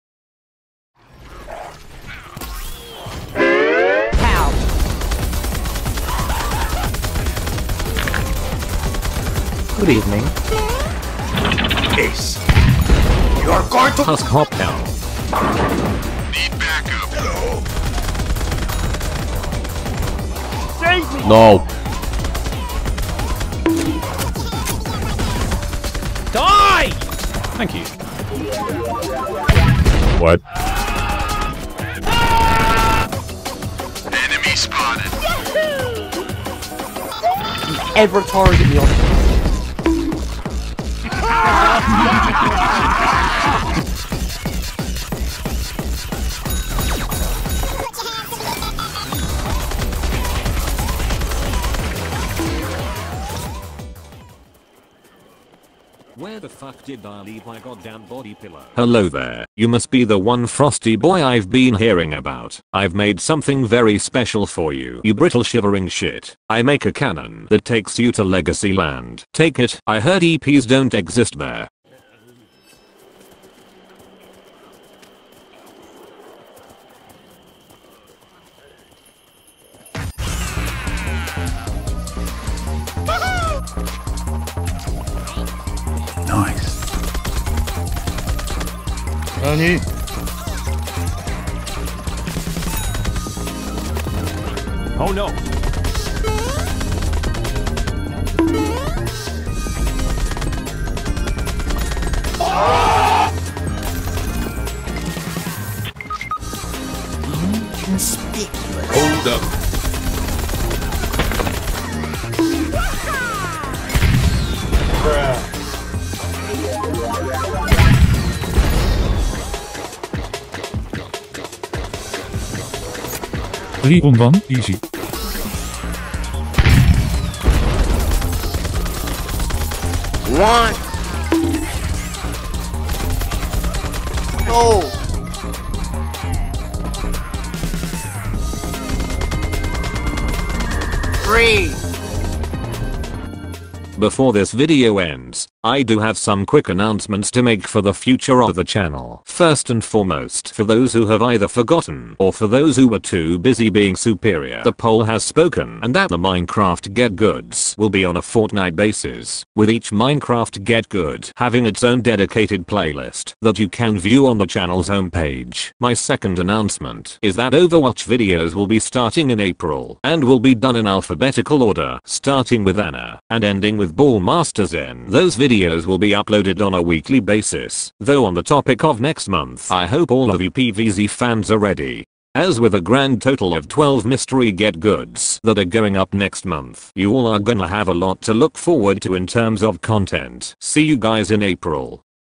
One frosty Boi
A meme montage dedicated to the cheesiest soldier in the game, plus some updates.

0:00 meme stuff
2:24 announcement stuff

All music used can be found during the first 10 seconds it is playing, with its name being displayed in the bottom corner.